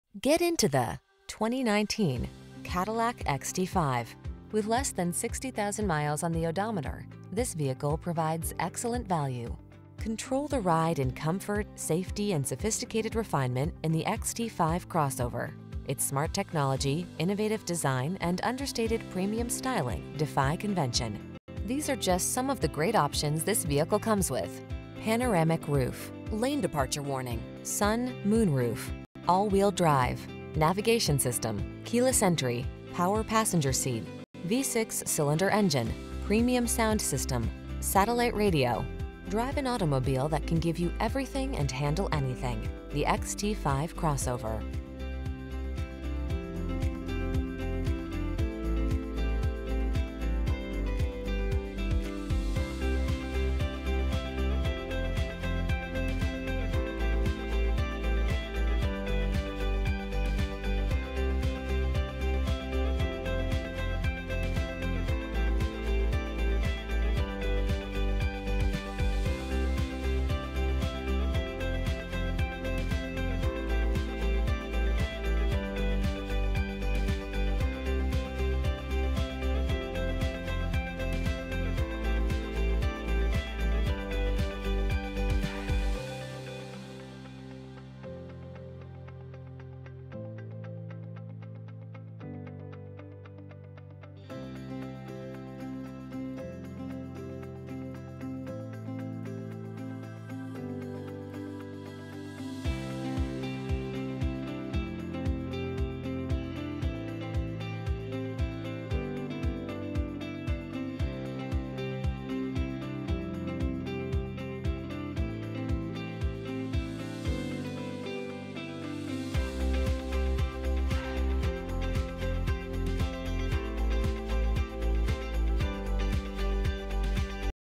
2019 Cadillac XT5 Natchez, Woodville, Gloster, MS, Vidalia, Ferriday, LA 120549
Radiant Silver Metallic Used 2019 Cadillac XT5 Premium Luxury AWD available in Natchez, Mississippi at Natchez CDJR. Servicing the  Woodville, Gloster, MS, Vidalia, Ferriday, LA area.

Natchez CDJR
201 Sergeant Prentiss Dr